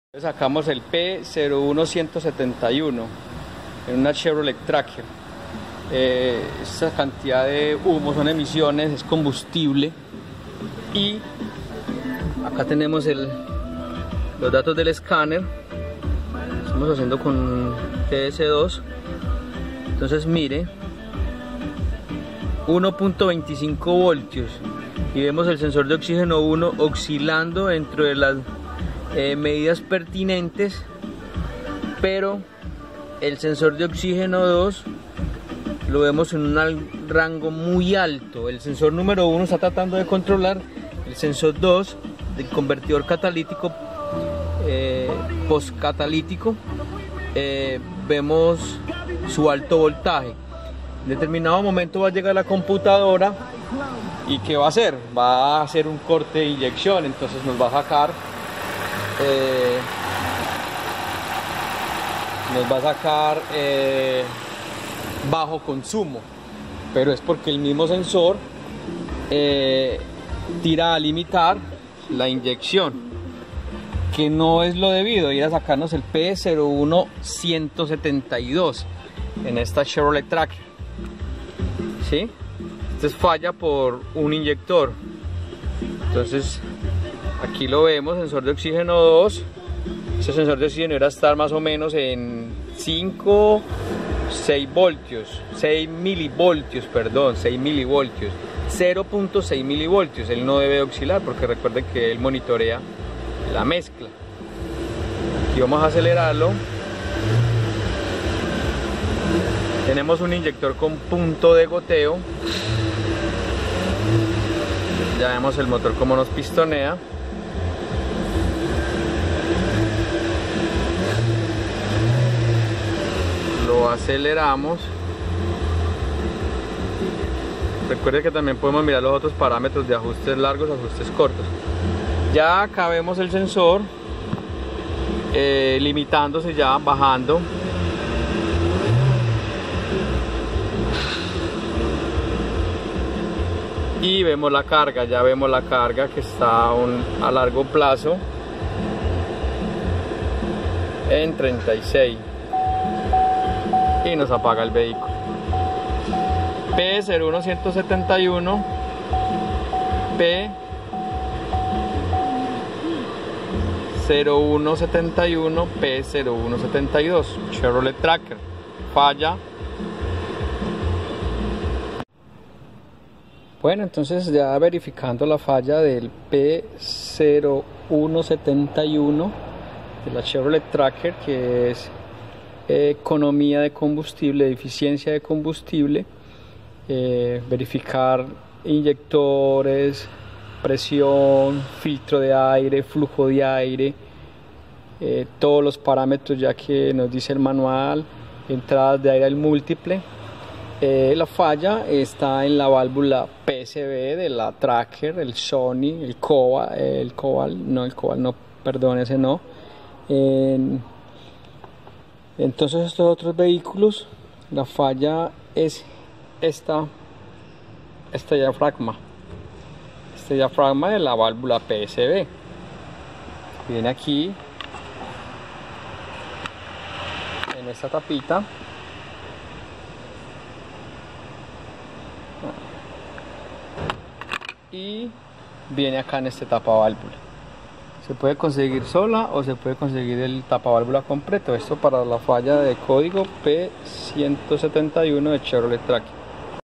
P0171 CÓDIGO FALLA CHEVROLET TRAKER
Falla del sistema por mezcla de combustible demasiado pobre,el cual es causado por la válvula PCV del sistema de ventilación positiva del cárter en los motores Ecotec de Chevrolet